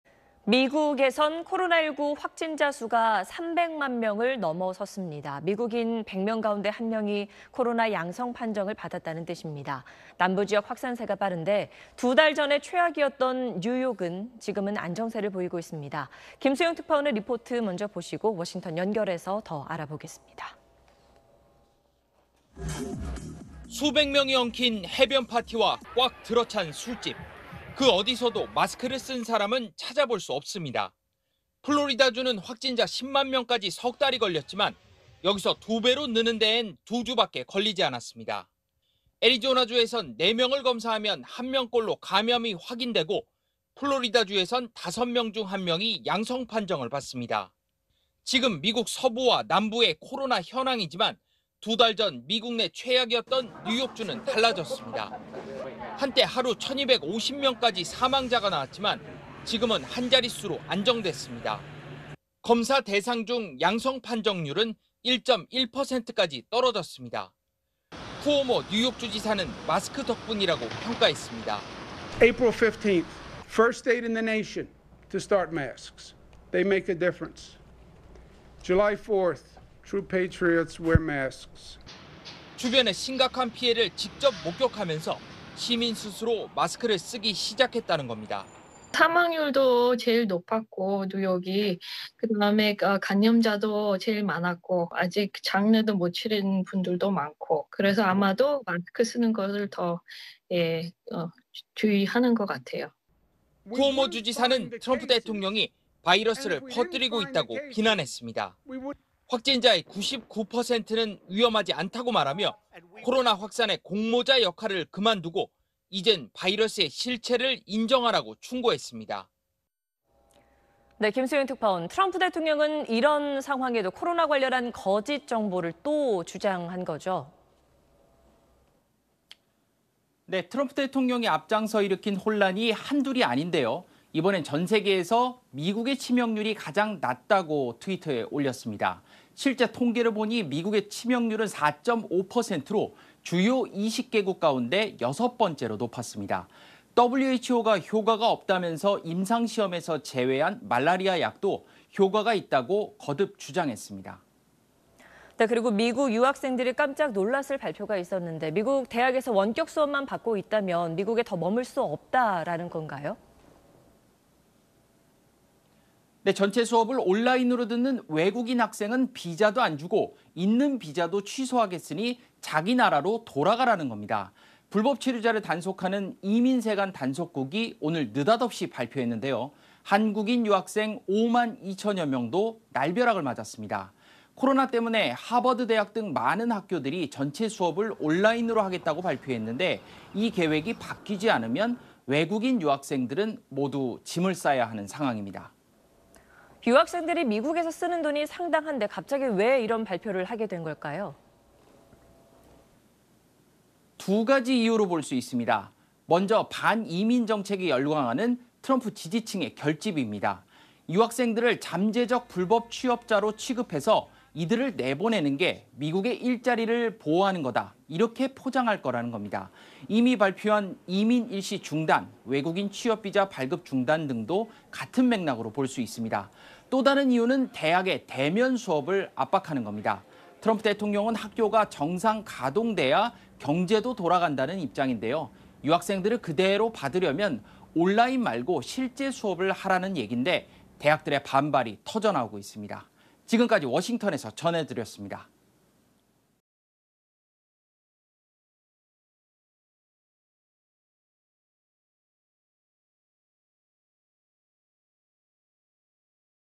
"마스크 덕분" 달라진 뉴욕…"트럼프가 코로나 확산" / SBS
미국에서는 코로나19 확진자 수가 300만 명을 넘어섰습니다. 미국인 100명 가운데 1명이 코로나 양성 판정을 받았다는 뜻입니다. 남부지역 확산세가 빠른데 두 달 전에 최악이었던 뉴욕은 지금은 안정세를 보이고 있습니다.
김수형 특파원 리포트 먼저 보시고 워싱턴 연결해서 더 알아보겠습니다.